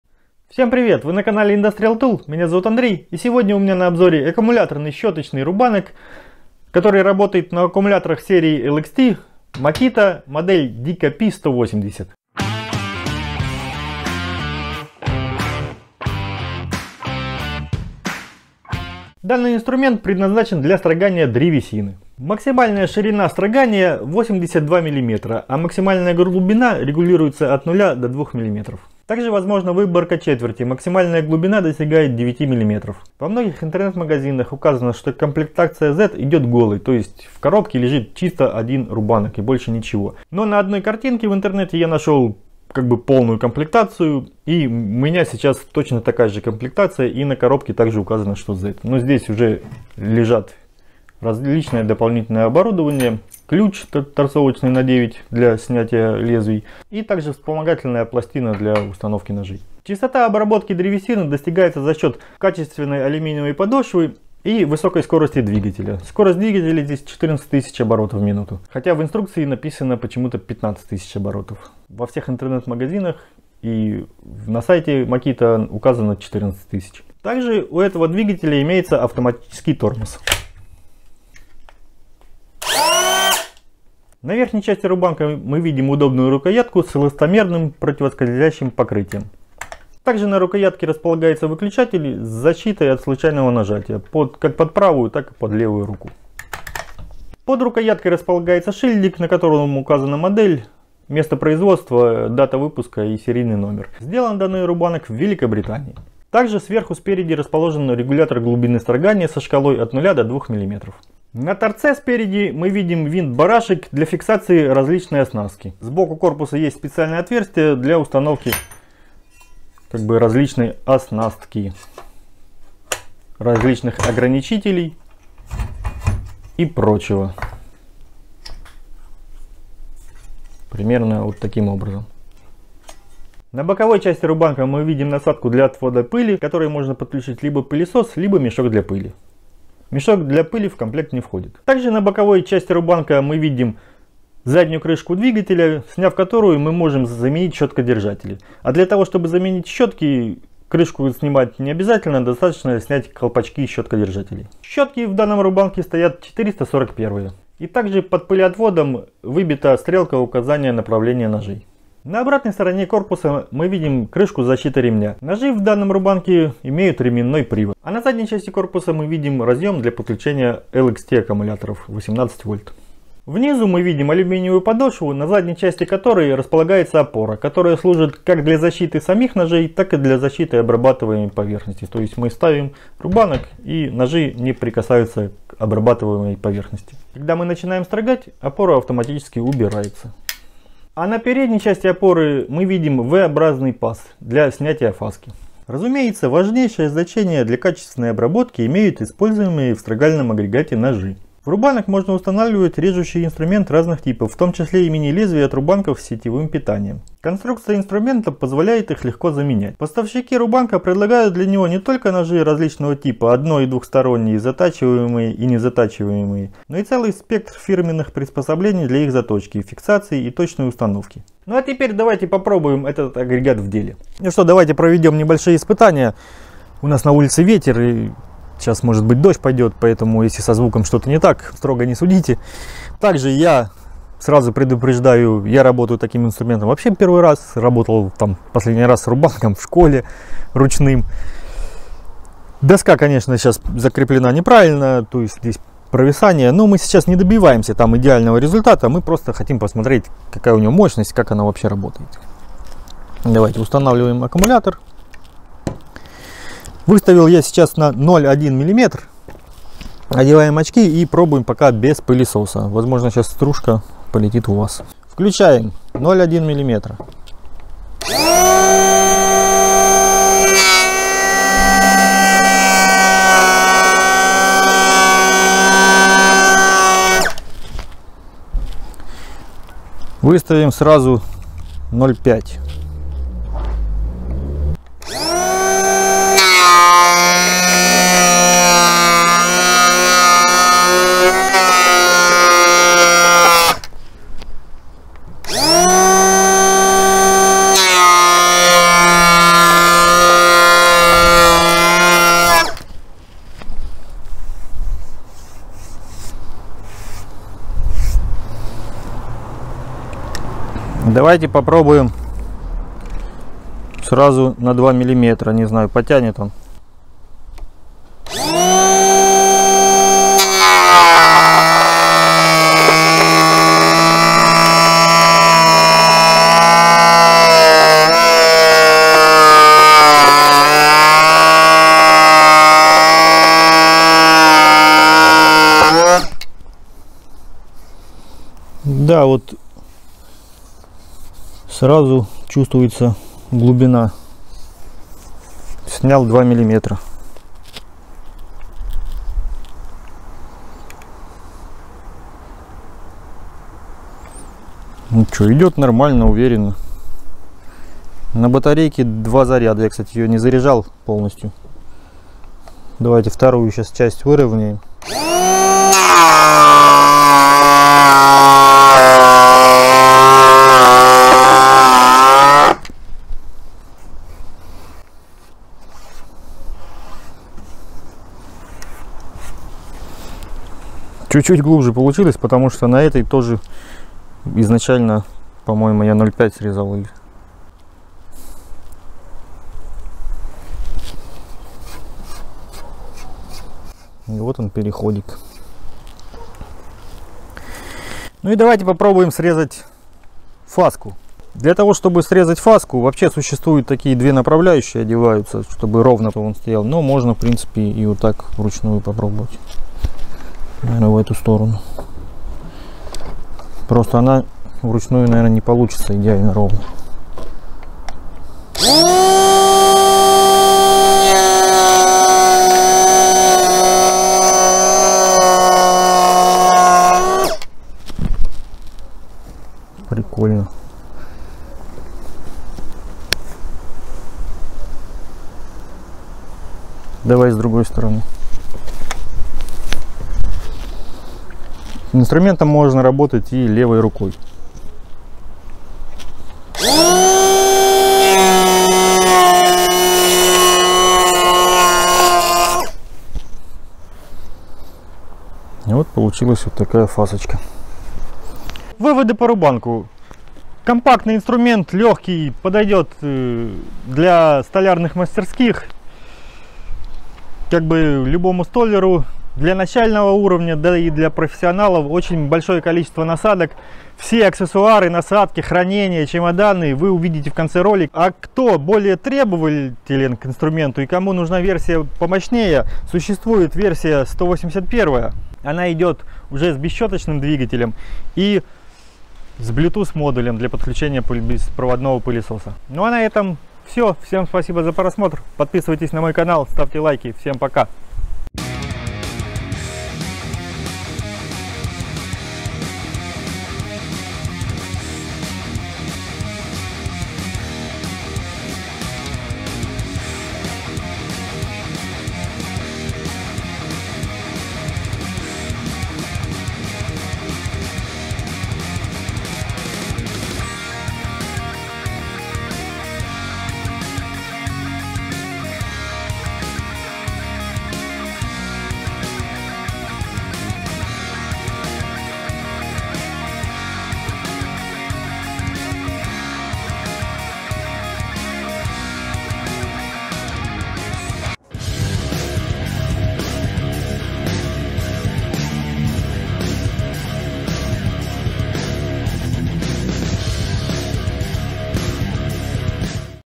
Аккумуляторный рубанок Makita DKP180Z
Универсальный инструмент для строгальных работ с шириной ножа 82 мм. Глубина строгания - до 2,5 мм, на фальц - до 9 мм, настройка с точностью до 0,1 мм. Литиево-ионный аккумулятор 18 В, в комплект поставки не входит.
Комплект поставки рубанка Makita DKP180Z ограничен до минимума – в него входит только сам инструмент. Однако, это не только маркетинговый ход, но и возможность мастеру подобрать для себя подходящий набор расходных материалов, с помощью которого этот агрегат можно превратить в универсальный строгальный станок. Поставщики рубанка предлагают потребителям широчайший ассортимент приспособлений, позволяющих выполнять практически все операции, связанные со строганием древесины, в том числе снятие фасок и фальцевание.
Максимальная глубина строгания на фальц рубанком Makita DKP180Z составляет 9 мм, а глубина обычного строгания ограничена величиной 2,5 мм при захвате полосы материала 8,2 см. Толщина снимаемой стружки при этом может изменяться с филигранной точностью в 100 мкм – для этого у агрегата имеется регулировочная рукоятка и легко читаемая шкала. Чистота обработки обеспечивается за счёт высокого качества алюминиевой подошвы, способствует этому и высокая скорость работы привода – 14 тыс. об/мин.
Разумеется, важнейшее значение для качественной обработки имеют используемые в строгальном агрегате ножи. В рубанок Makita DKP180Z можно устанавливать режущий инструмент разных типов, в том числе и мини-лезвия от рубанков с сетевым питанием. Ширина лезвия составляет 82 мм, и конструкция инструмента позволяет их легко заменять. Поставщики рубанка предлагают для него не только ножи различного типа (одно- и двухсторонние, затачиваемые и не затачиваемые), но и целый спектр фирменных приспособлений для их заточки, фиксации и точной установки.
Конструкция рубанка Makita DKP180Z полностью соответствует традициям, свойственным продукции производства Макита. Агрегат весит всего 3,4 кг и при длине 33,3 см прекрасно сбалансирован и легко удерживается за счёт эластомерного покрытия рукоятки и её эргономичной формы. Мощности привода рубанка вполне хватает для строгания твёрдой древесины, ведь питание осуществляется от литиево-ионного аккумулятора с повышенным напряжением 18 В.
Электродвигатель у инструмента Makita DKP180Z щёточный и оснащён электрическим тормозом, так что вращение лезвия после отпускания выключателя прекращается мгновенно. Кроме того, оно защищено от повреждений автоматически выдвигающейся подставкой в основании подошвы рубанка. Для снятия фасок в её передней части имеется V-образная выемка.
Имеется у рубанка Makita DKP180Z и возможность присоединения пылесоса, для этого в правой части имеется отверстие для переходника. Сюда же можно установить мешок для сбора пыли. При необходимости выполнения серийных операций по обработке большого количества одинаковых заготовок можно порекомендовать ещё приобретение различных направляющих, которые обеспечат идеальную повторяемость операций.
Таким образом, инструмент Makita DKP180Z - это базовый элемент, на основе которого можно скомплектовать профессиональный набор для любых строгальных работ.